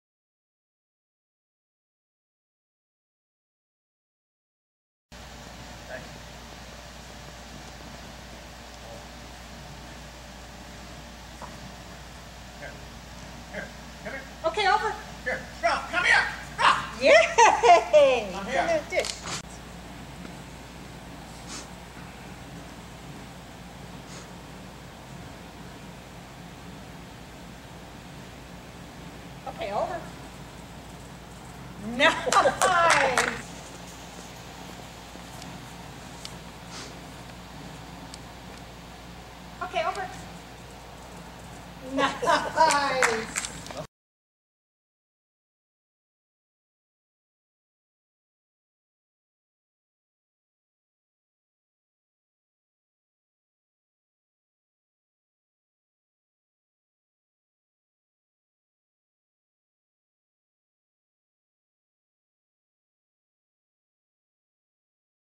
Willie's First Jump Chutes
Much to my amazement, Willie did the jumps chutes with no problems.